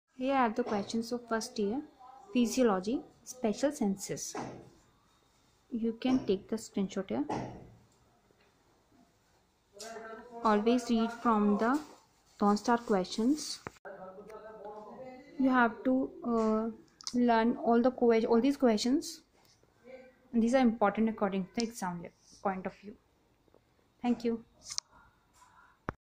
Special senses - Physiology important questions for first year mbbs student..
Pathways of special senses- eyes ,nose, ear and tounge are important both for viva and written exam..